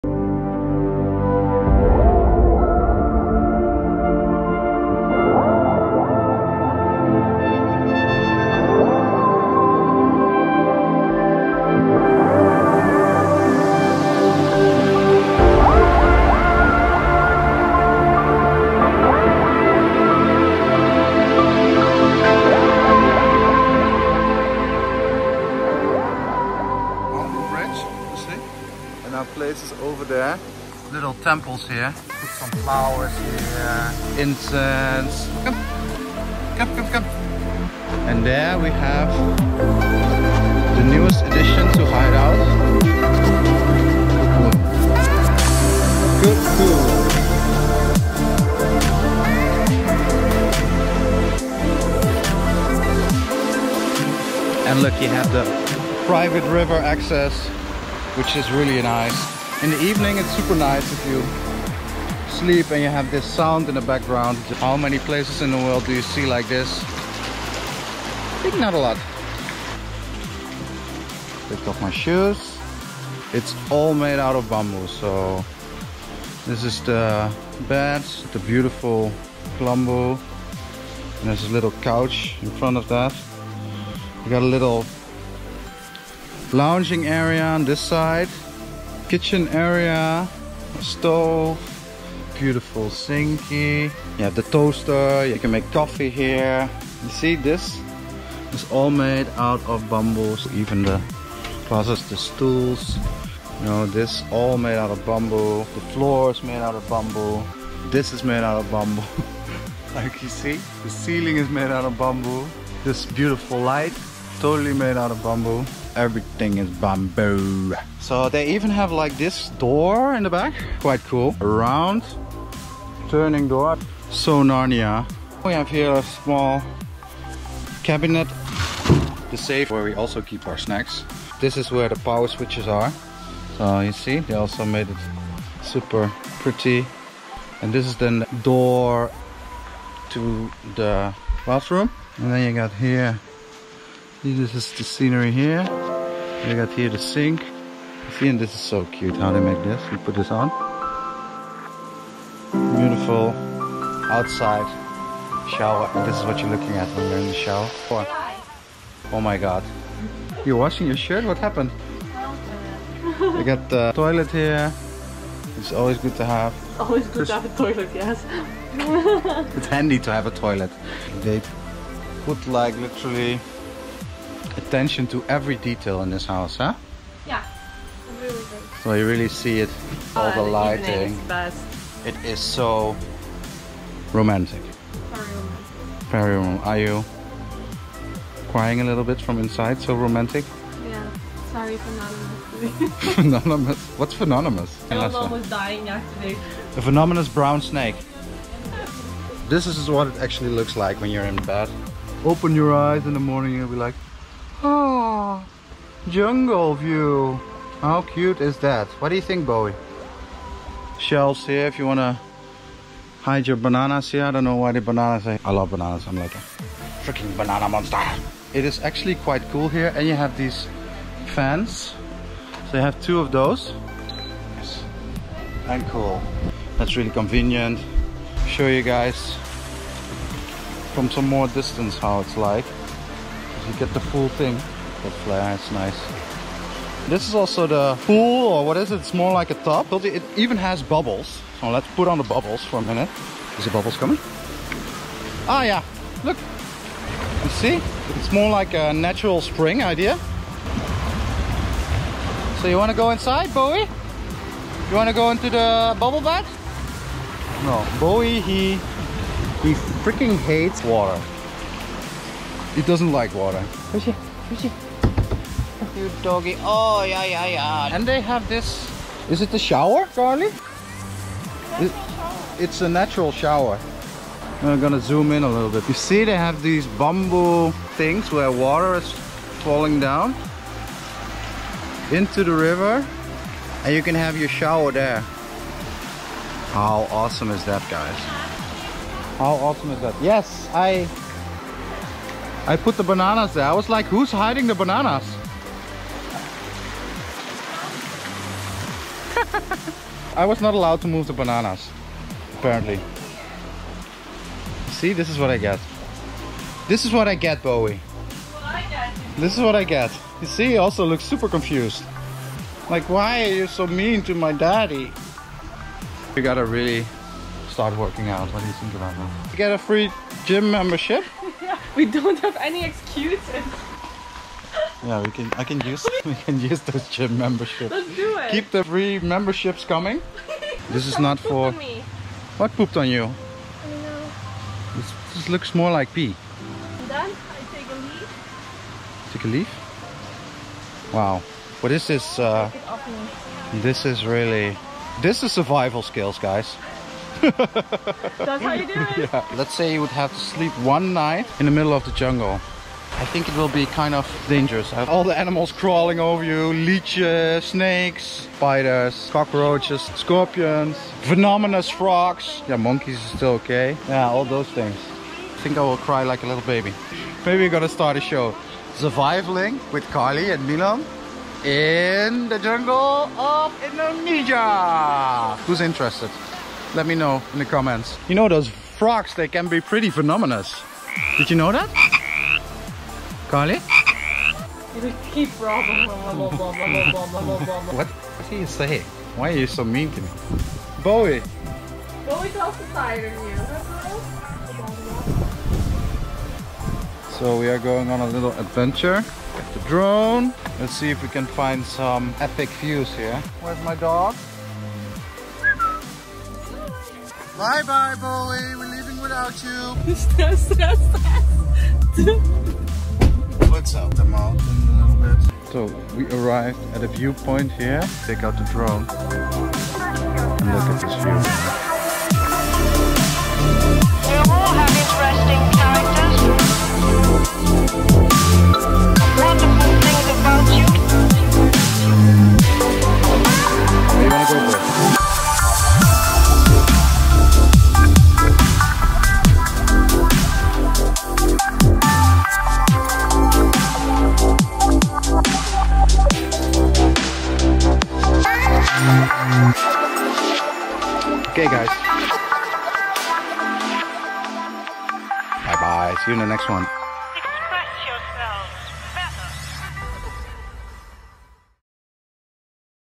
BALI'S MOST UNIQUE VILLA'S EPISODE #1✨THE NEWEST VILLA AT HIDEOUT BALI...COCOON! 😁❤️
We had such an amazing time in this beautiful place in the jungle of Bali. This should be on your list of go to places when you visit Bali.